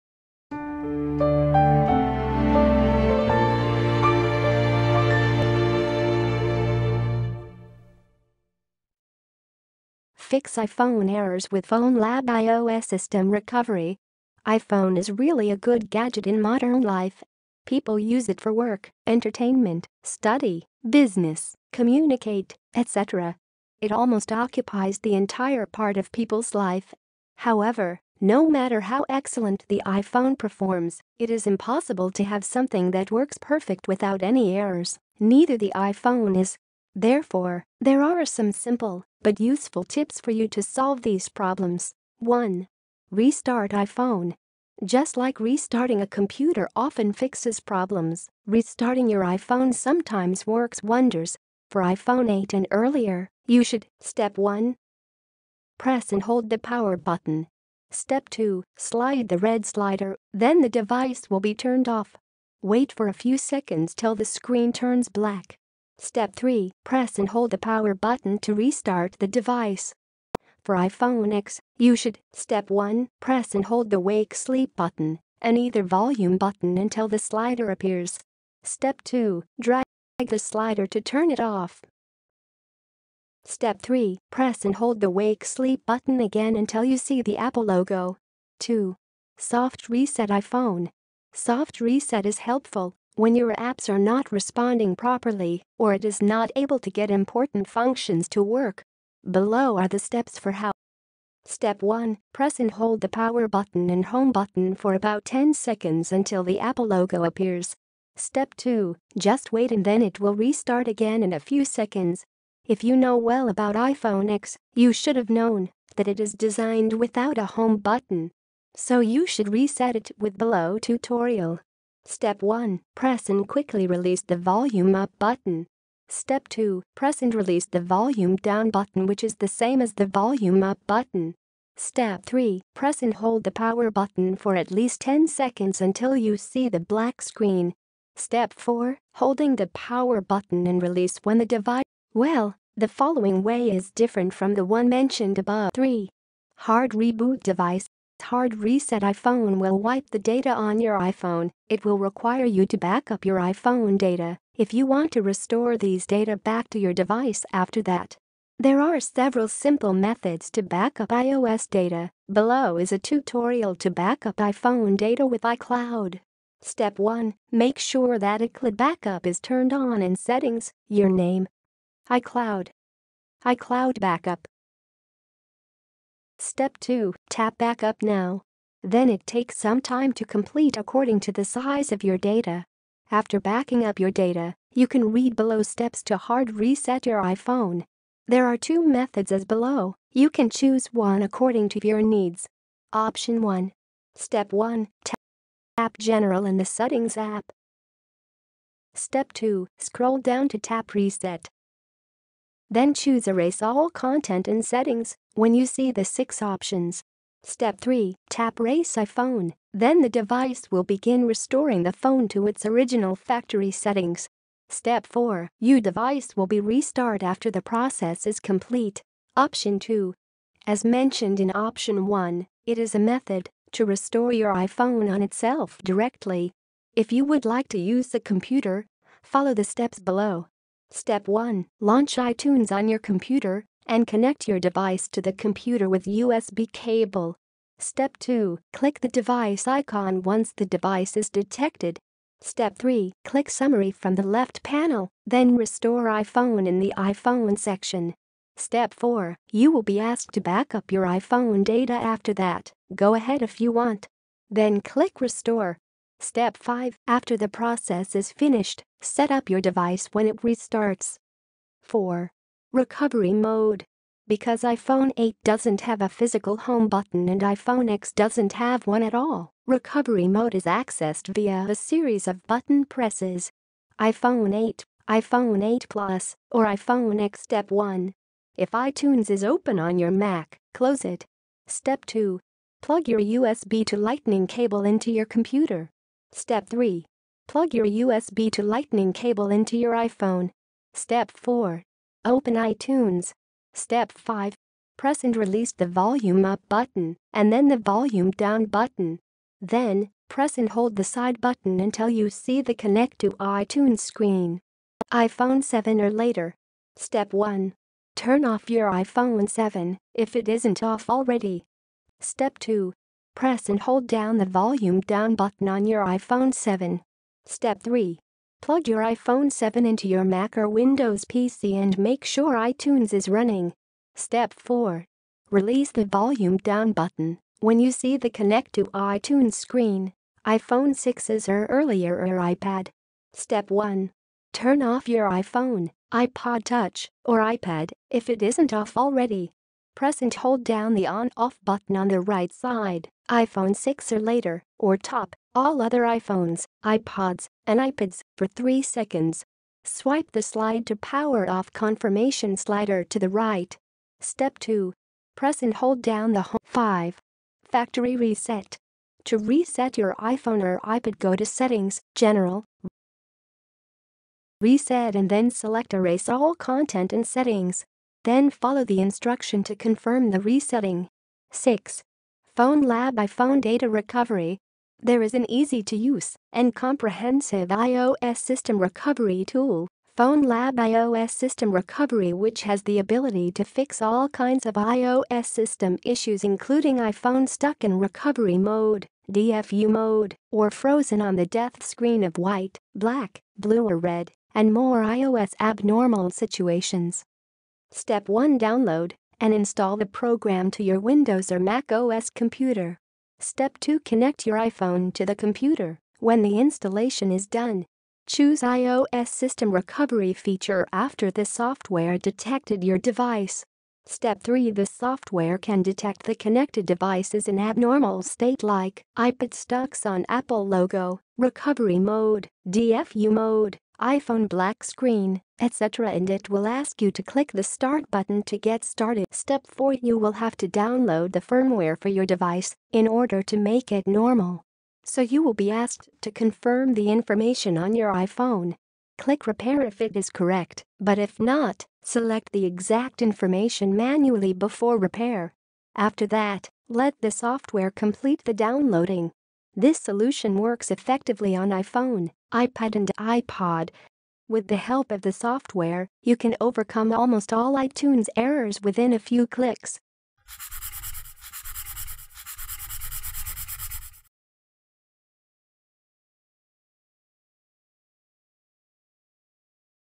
Fix iPhone Errors with FoneLab iOS System Recovery - 2022
This is a video about how to fix iPhone errors with effective methods. You can also visit the post for more details.
If you want to know how to fix iOS errors through restart iPhone, reset iPhone or use the iPhone system recovery, you can view this video to get some useful tips.